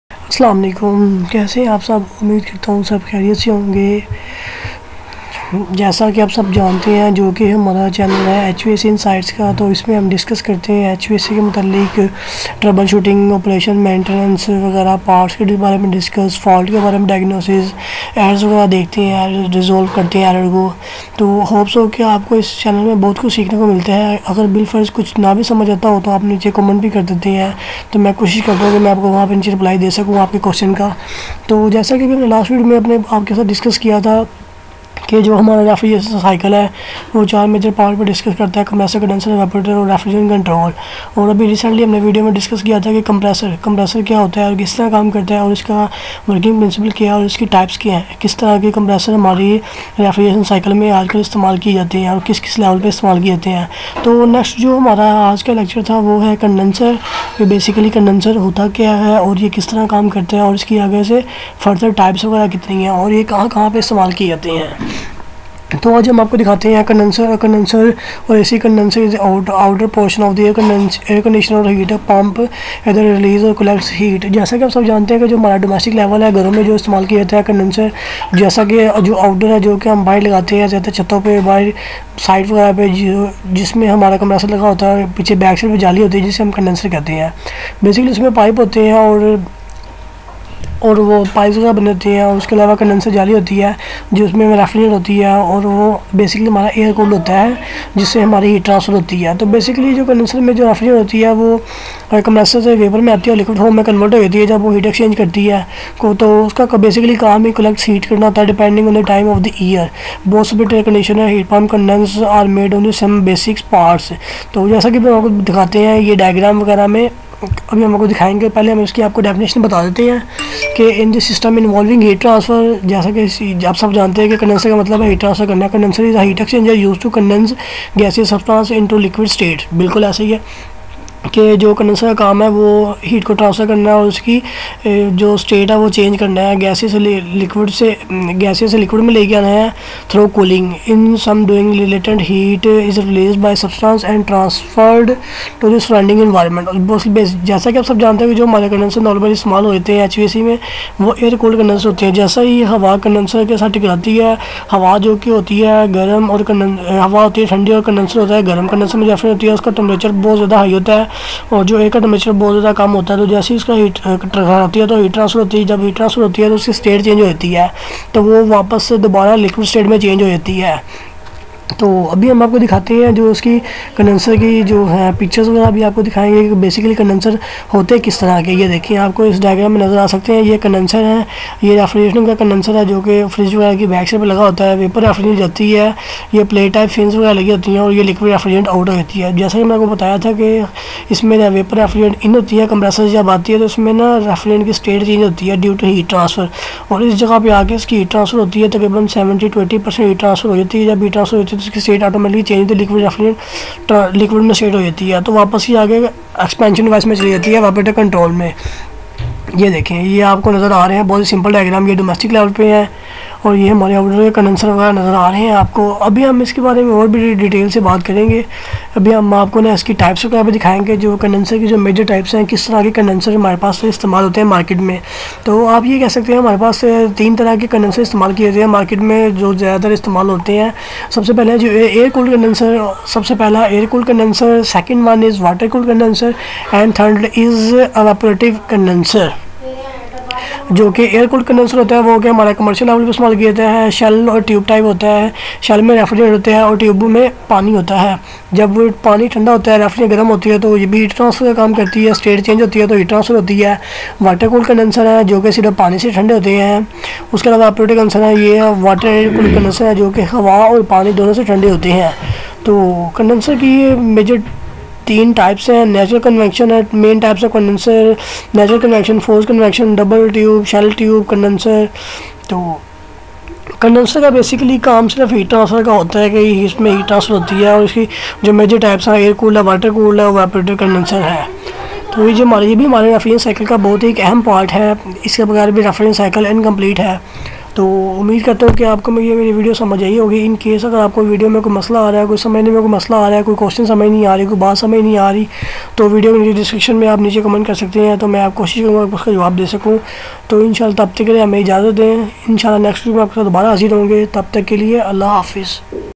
Explain refrigerant Condenser And its all types with working Principle
Explain refrigerant Condenser And its all types with working Principle air cooled condenser water cooled condenser evaporation Condenser